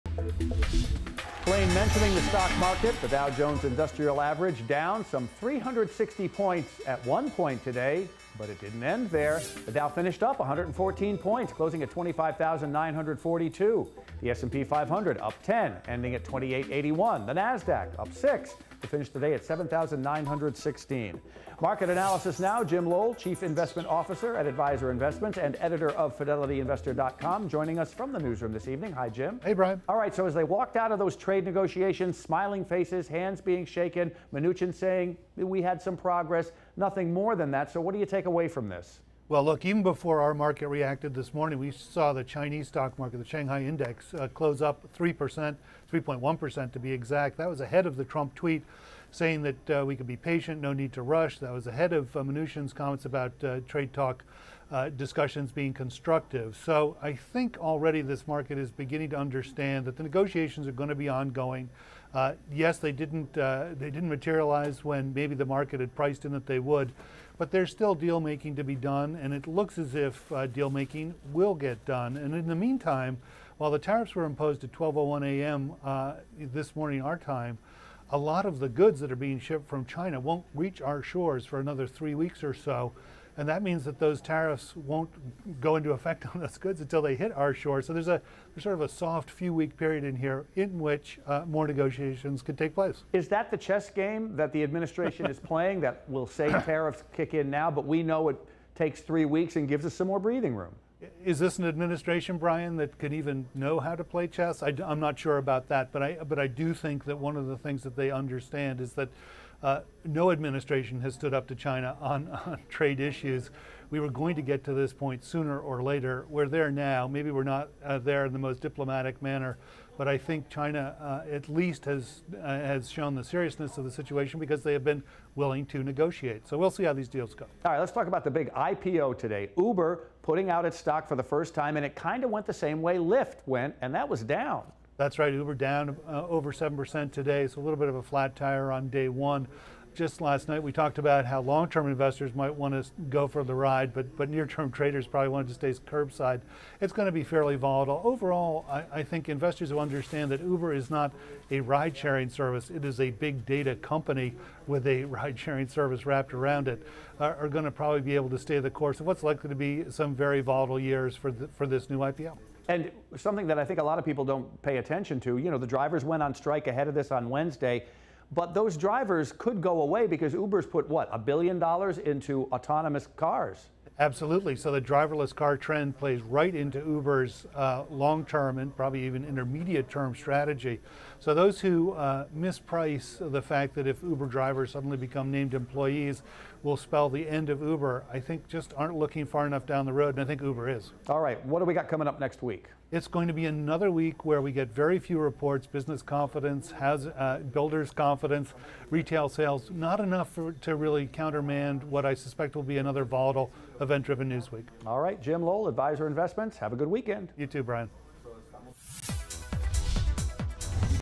U.S. Indexes Advance as U.S.-China Trade Talks Continue
Adviser Investments Chief Investment Officer Jim Lowell has the market analysis for Friday, May 10. All major U.S. indexes closed the day with slight gains to reverse a five-day losing streak. The Dow Jones Industrial Average and S&P 500 rose 0.4% and the and NASDAQ Composite gained 0.1%. Similarly, the Shanghai Composite Index advanced 3% as investors in both the U.S. and China expressed optimism that trade negotiations are continuing—despite the 12:01 a.m. tariff increase on $200 billion of Chinese goods. Meanwhile, Uber launched its much-anticipated I.P.O. as its stock fell more than 7% in intra-day trading, with the company’s valuation falling from $75.5 billion to $69.7 billion.